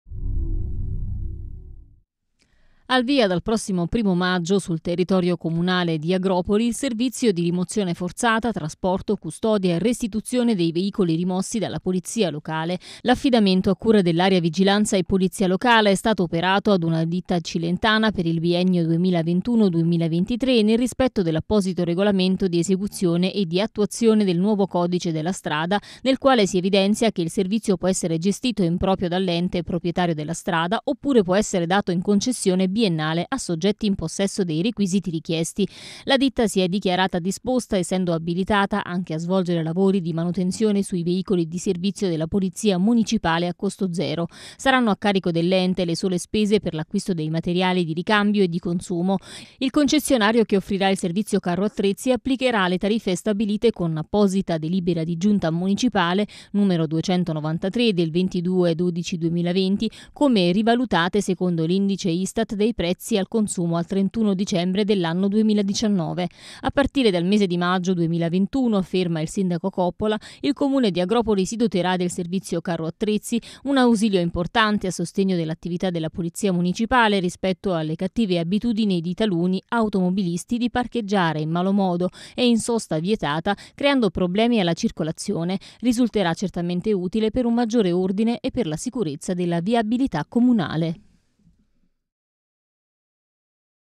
Agropoli, Al via il servizio di rimozione forzata in città
Al via dal prossimo 1 maggio 2021 sul territorio del Comune di Agropoli il servizio di rimozione forzata, trasporto, custodia e restituzione dei veicoli rimossi dalla polizia locale.